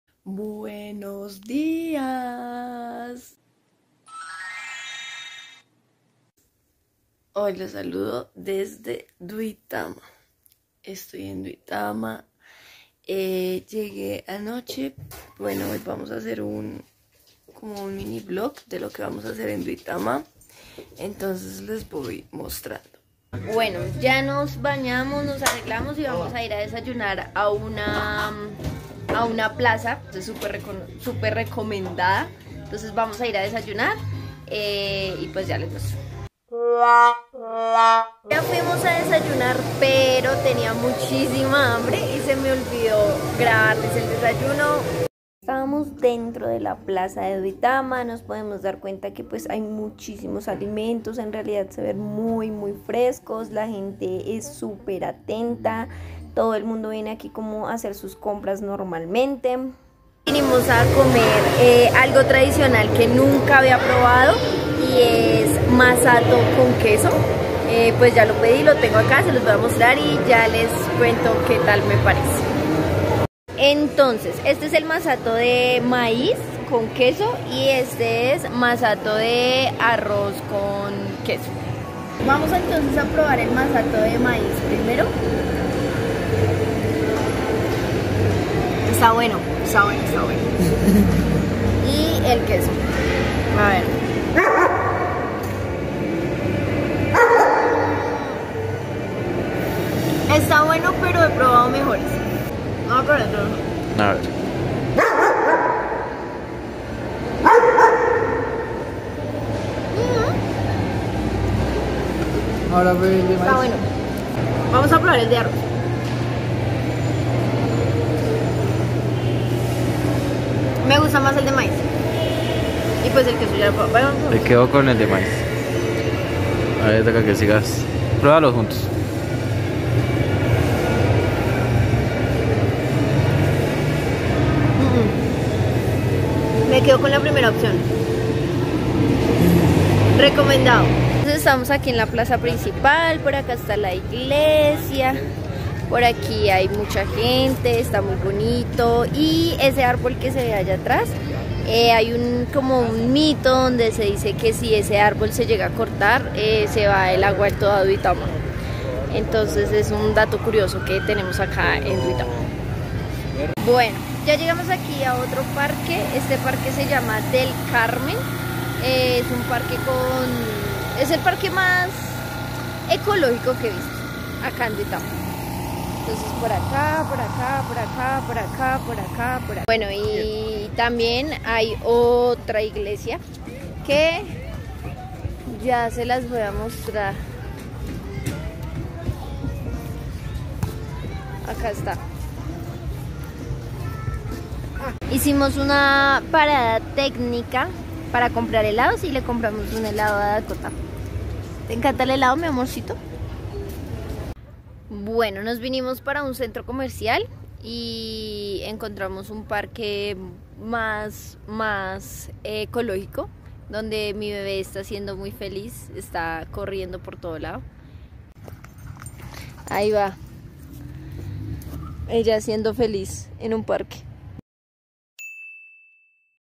Viaje a Duitama parte 1🍀
Me fui de viaje para Duitama - Boyacá y quiero que me acompañen en esta aventura. 🥰
También fue algo nuevo ya que este pueblo no lo conocía y es muy bonito, esta es la primera parte.
Espero que les guste y no se pierdan la segunda parte de este video. ✌️